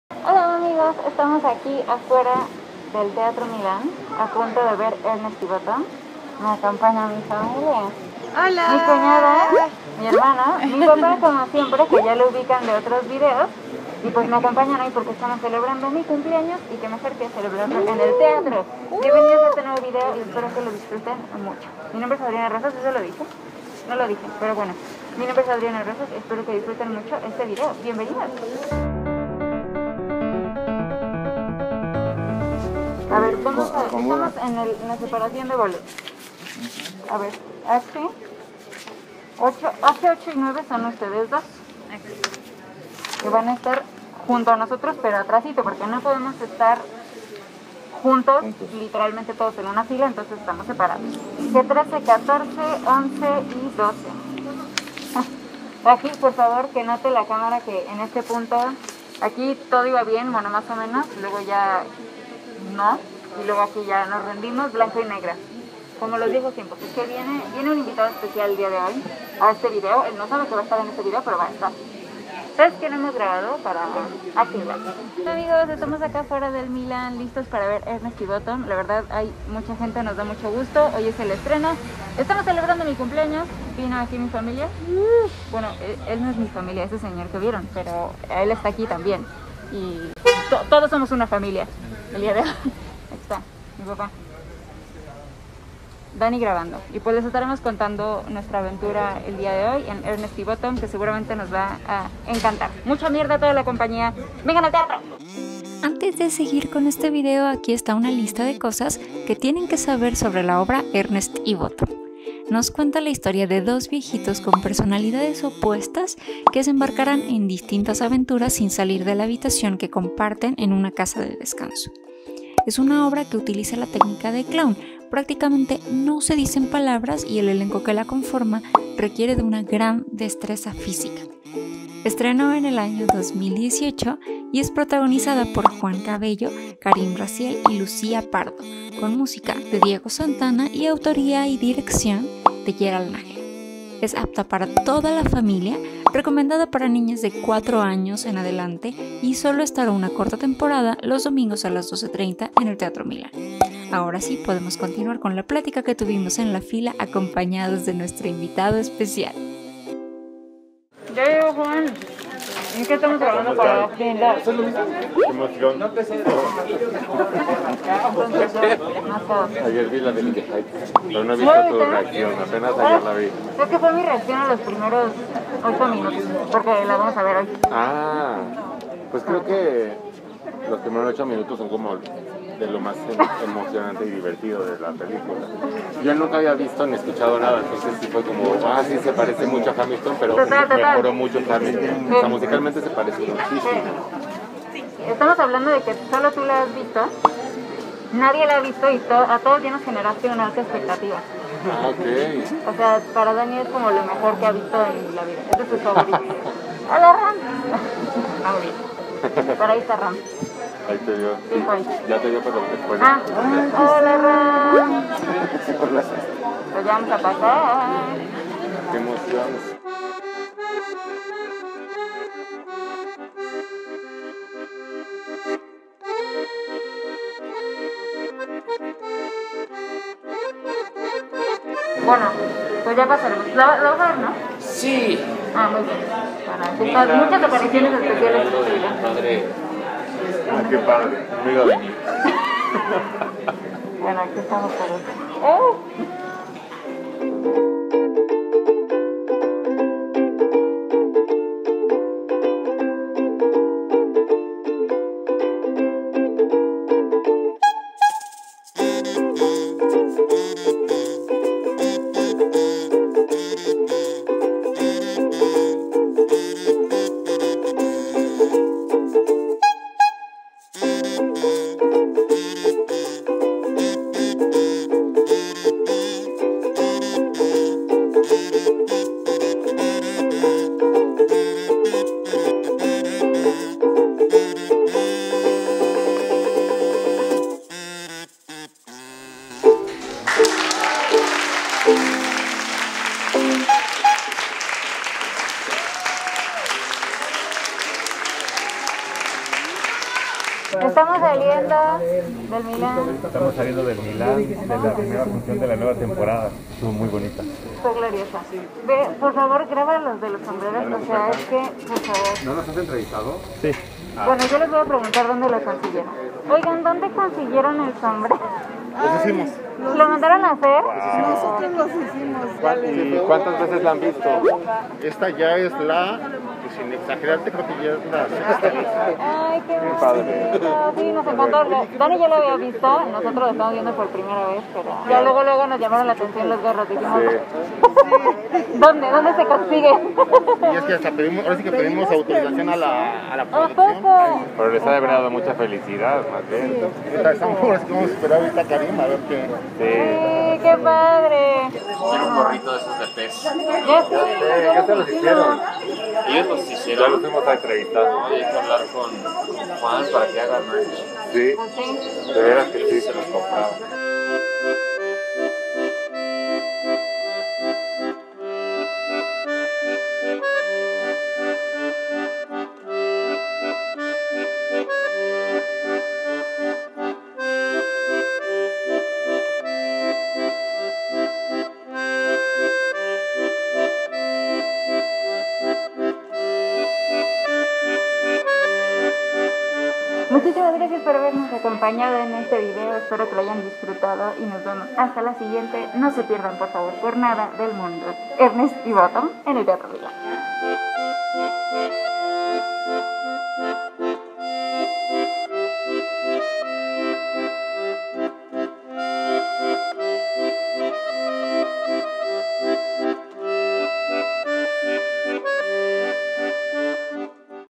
Esto sucedió en el estreno de la obra Ernest & Bottom 🎁
El pasado 20 de junio se estrenó en el Teatro Milán la nueva temporada de la obra Ernest & Bottom, producida por Nocturno Teatro, y estuvimos ahí junto a todo el público que fue testigo de esta hermosa historia.

CONOCE TODAS NUESTRAS RECOMENDACIONES DE TEATRO Y ADQUIERE TUS BOLETOS AQUÍ ➡️